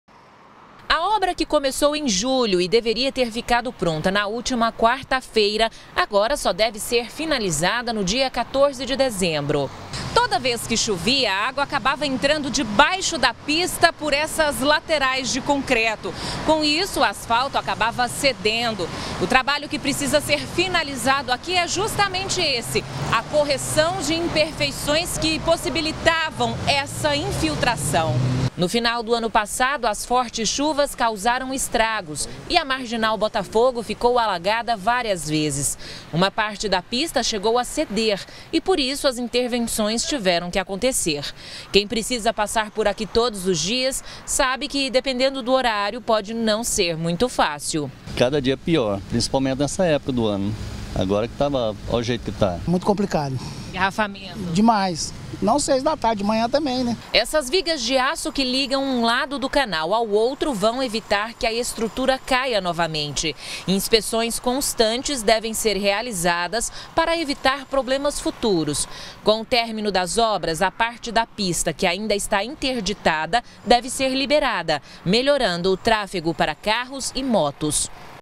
MARGINAL BOTAFOGO: AVENIDA VAI SER LIBERADA NO FIM DA SEMANA QUE VEM
A conclusão das obras na Marginal Botafogo foi prorrogada para o fim da semana que vem. O motorista deve ficar atento porque o tráfego em alguns pontos da avenida  está permitido em apenas meia pista. Os transtornos continuam ainda por mais alguns dias.